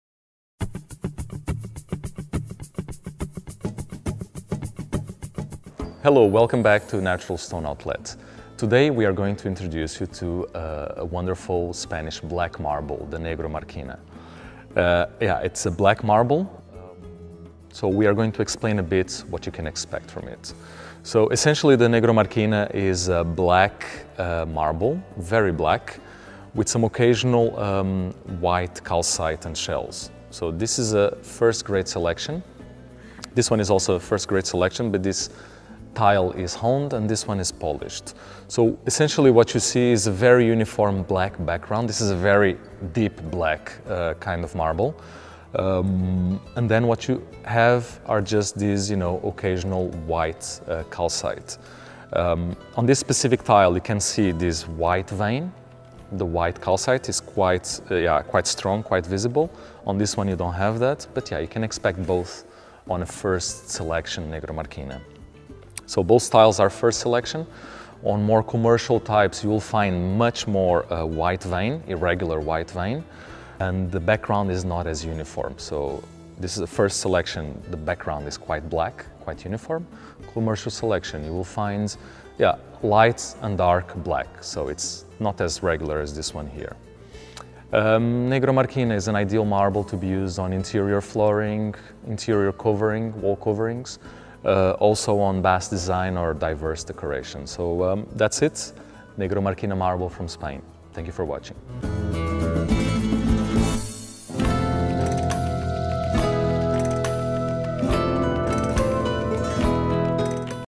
How to Choose Negro Marquina marble
The Negro Marquina is a fairly unique black marble from Spain, showing a deep black color and some occasional dramatic white vein. On this video, we make a quick introduction to this marble, giving some information about how to choose it.